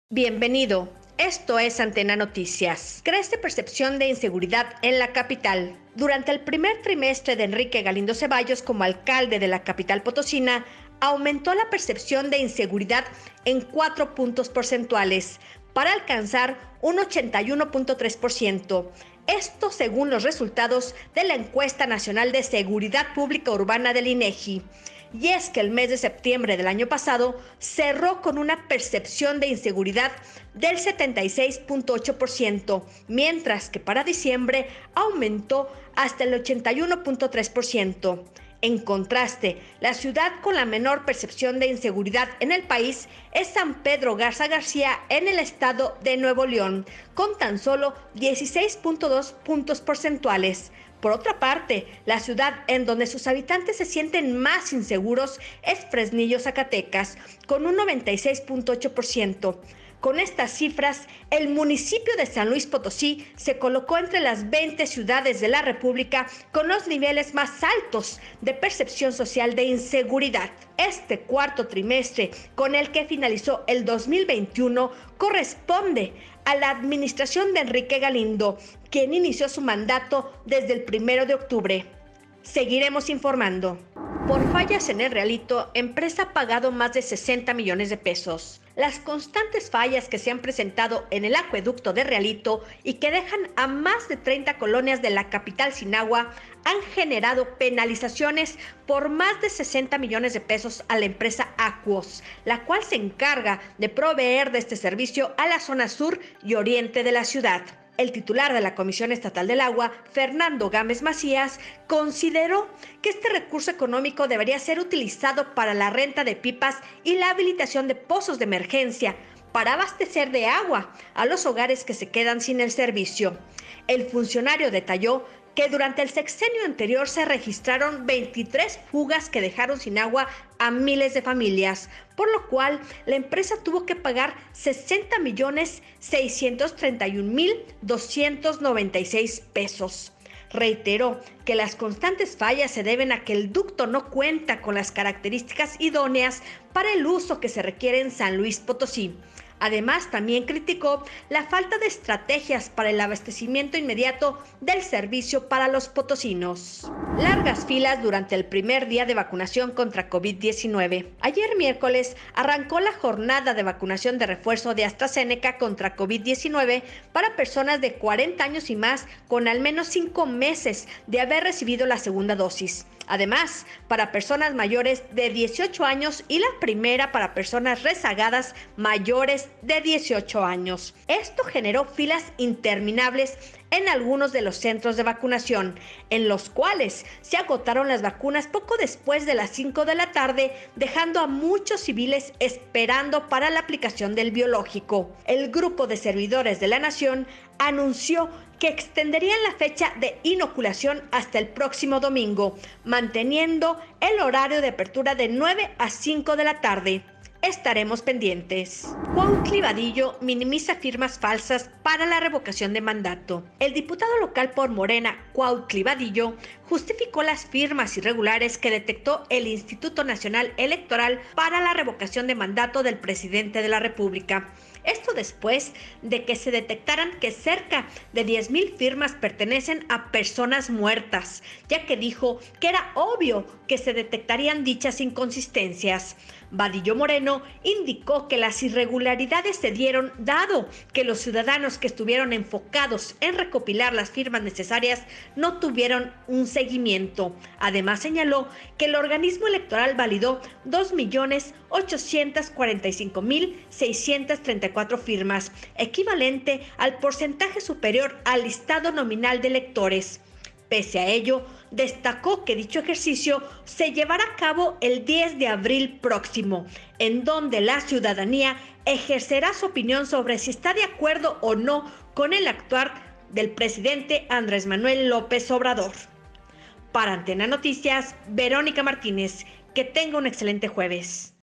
🚨 ＡＮＴＥＮＡＮＯＴＩＣＩＡＳ 🚨 ◼️Aquos ha pagado más de 60MDP por las fallas en El Realito
◼️ Diputado Local minimiza firmas falsas para encuesta de revocación de mandato

◼️Aquos ha pagado más de 60MDP por las fallas en El Realito

◼️ Capitalinos se sienten más inseguros durante el mandato de Enrique Galindo